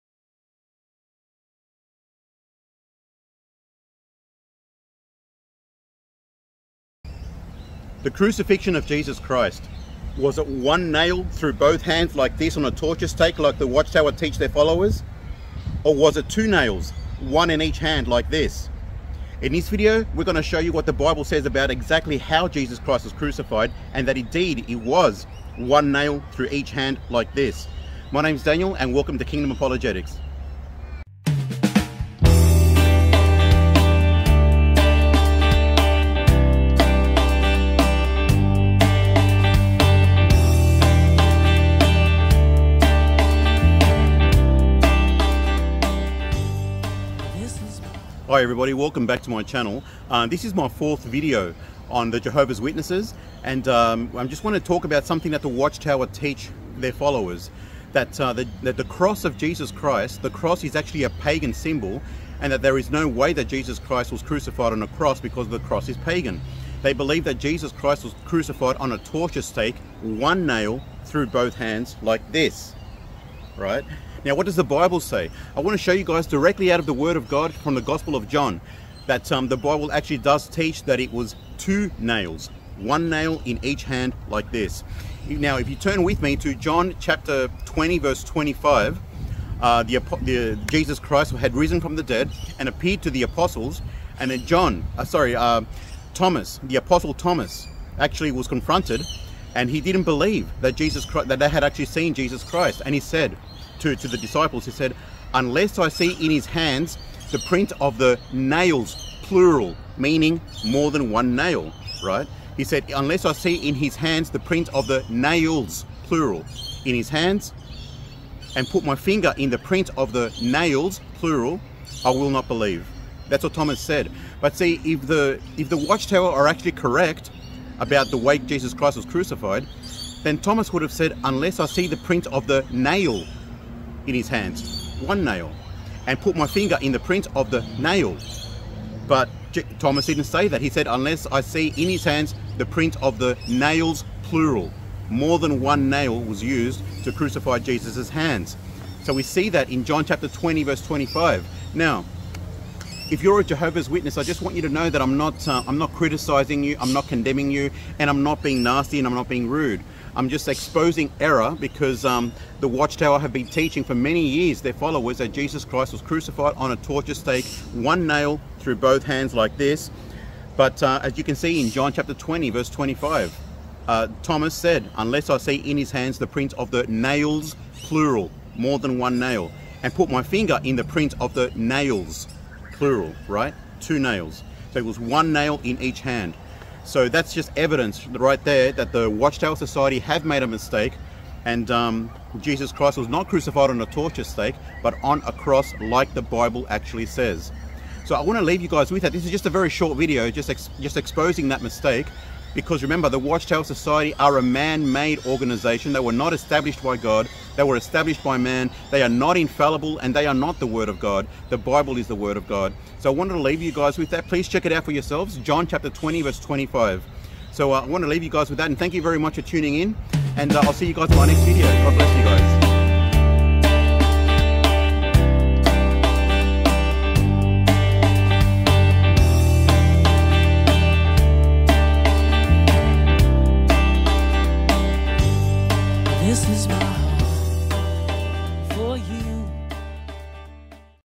The Watchtower Dilemma (Part 4/5)
In this video we will look at the Watchtower's version of the crucifixion vs The what the bible says about the crucifixion.